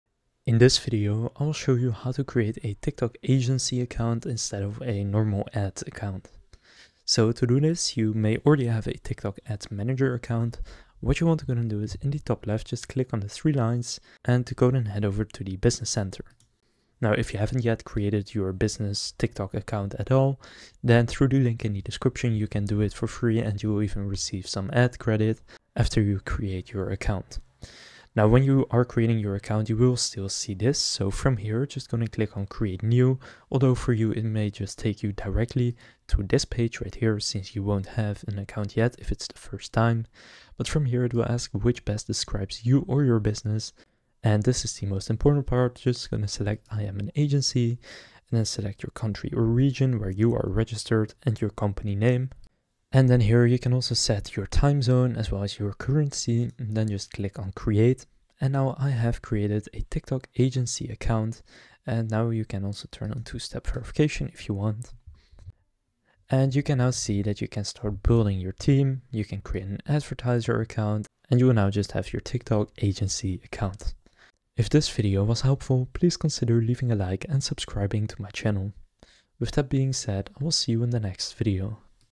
How To Create TikTok Agency Account - Step by Step
In this video I'll show you How To Create TikTok Agency Account.